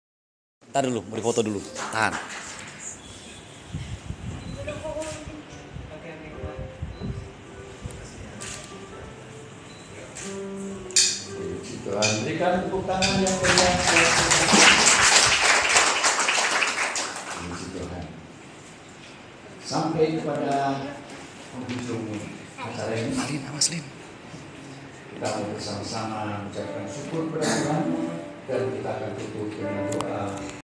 Video 3. The Wedding of Arlen Bernadus Gawa and Yolenda Mandolang-Pondaag
Video 3. The Wedding of Arlen Bernadus Gawa and Yolenda Mandolang-Pondaag || On December 9, 2017 (on Saturday) || Videographer :  Blacktator || Pastor : Rev. Yudo Wahono, S.Th